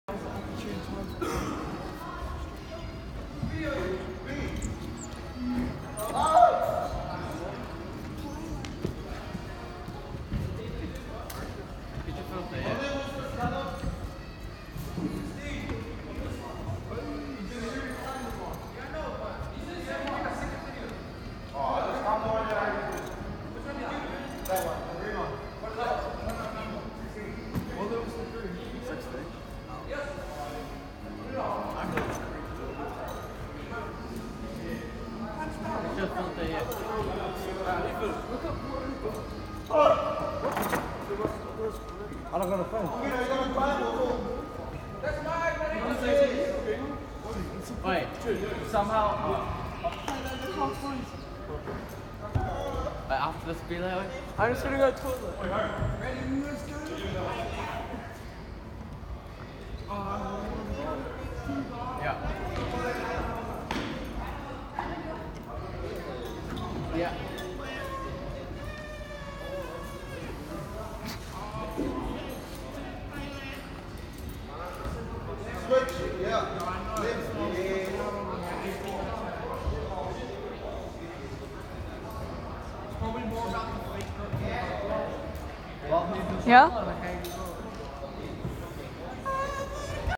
9 Year old climbs with NO HARNESS and FALLS **WARNING GRAPHIC**
warning video graphic R16

pray for this boy and his family